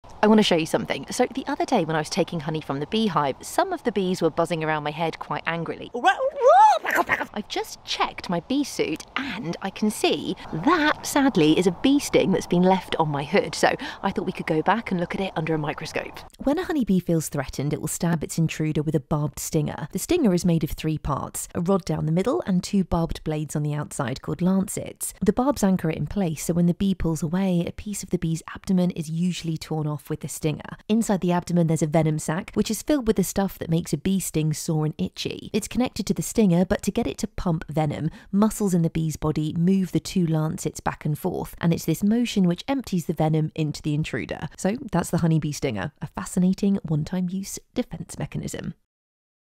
What happens when a bee stings you? | Maddie Moate
If you want to find out more about why honey bees sting then you can watch my full video!